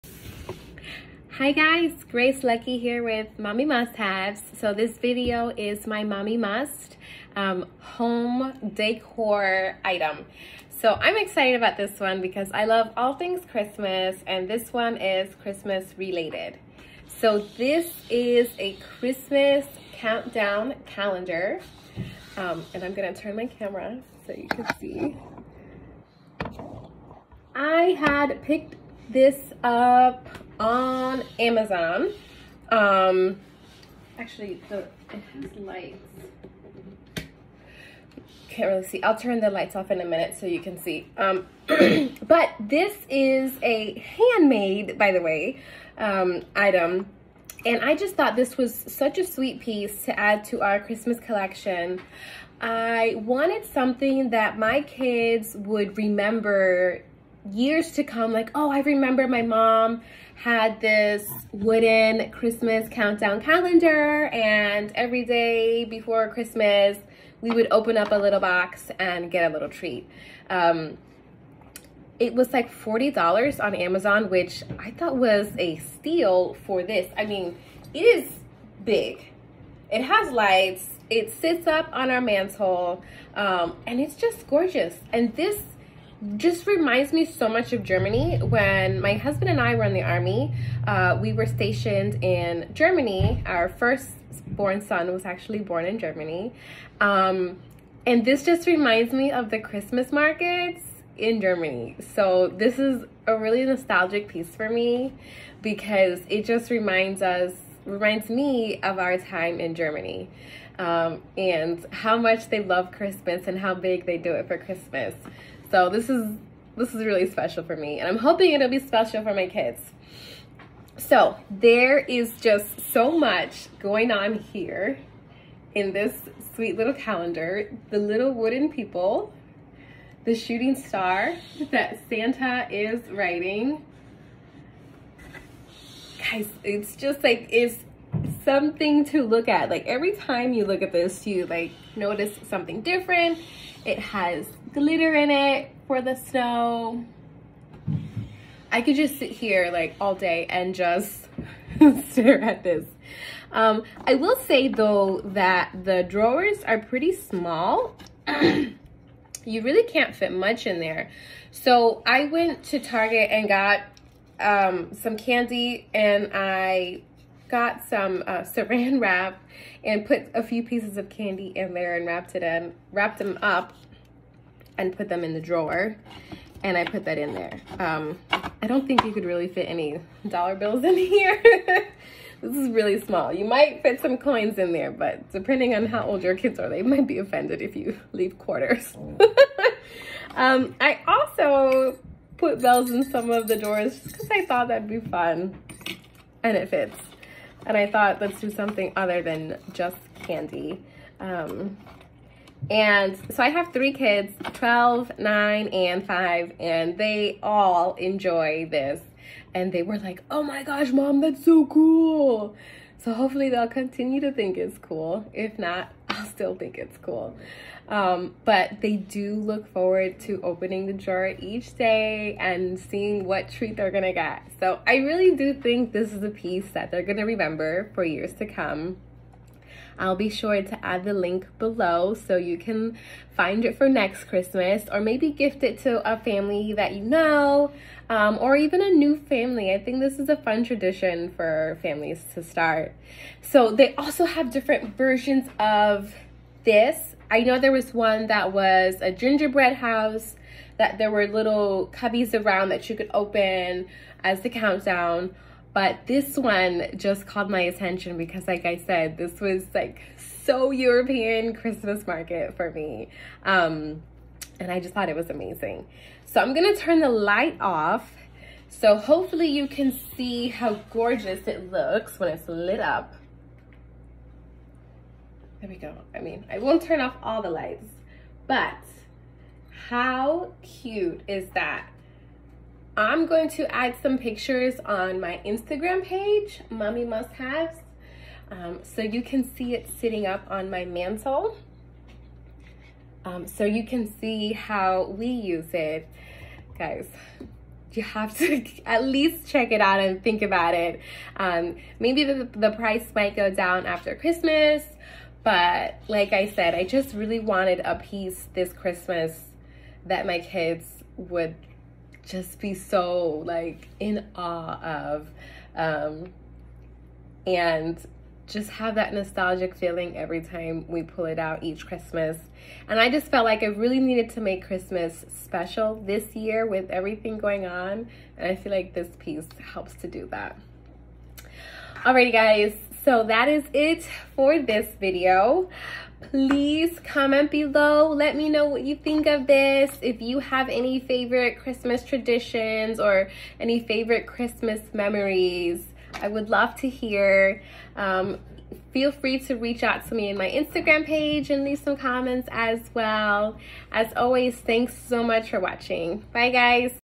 December Mami Must-Have Home Decor Product Review
This month I’m reviewing a Christmas Advent Calendar from Amazon. This piece makes a great addition to anyone’s Christmas collection. Definitely a winner with the kids. It brings back memories of living in Germany with my husband. Here is the link: